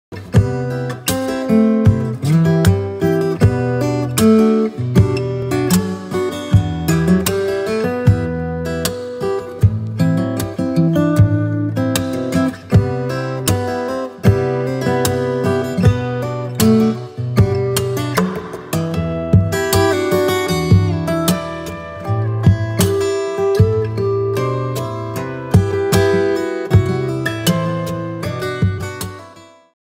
Headpiece - Headdress with violin in blue - creme colour & feathers
Handmade headdress for Lifeball 2018 - The sound of music